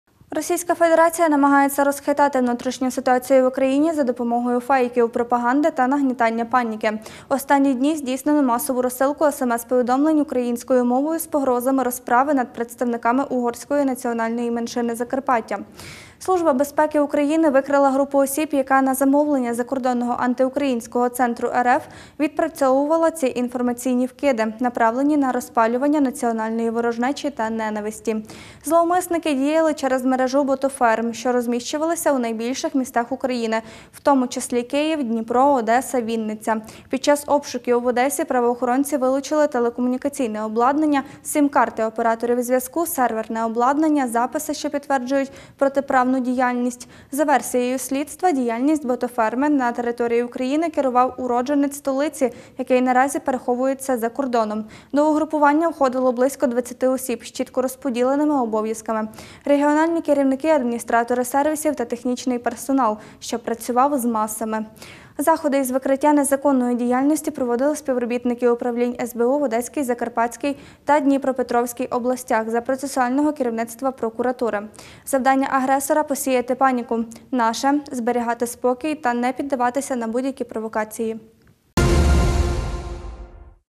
Відвернення провокацій. Робота СБУ
РФ намагається розхитати внутрішню ситуацію в Україні за допомогою фейків, пропаганди та нагнітання паніки. Останні дні здійснено масову розсилку смс-повідомлень українською мовою з погрозами розправи над представниками угорської національної меншини Закарпаття. Служба безпеки України викрила групу осіб, яка на замовлення закордонного антиукраїнського центру РФ, відпрацьовувала ці інформаційні «вкиди», направлені на розпалювання національної ворожнечі та ненависті. Зловмисники діяли через мережу ботоферм, що розміщувалися у найбільших містах України, у тому числі: Київ, Дніпро, Одеса, Вінниця. Під час обшуків в Одесі правоохоронці вилучили телекомунікаційне обладнання, сім-картки операторів зв’язку, серверне обладнання, записи, що підтверджують протиправну діяльність. За версією слідства, діяльністю ботоферми на території України керував уродженець столиці, який наразі переховується за кордоном. До угруповання входило близько 20 осіб, з чітко розподіленими обов’язками: регіональні керівники, адміністратори сервісів та технічний персонал, що «працював» з «масами». Заходи із викриття незаконної діяльності проводили співробітники Управлінь СБУ в Одеській, Закарпатській та Дніпропетровській областях за процесуального керівництва прокуратури. Завдання агресора – посіяти паніку, наше – зберігати спокій та не піддаватися на будь-які провокації.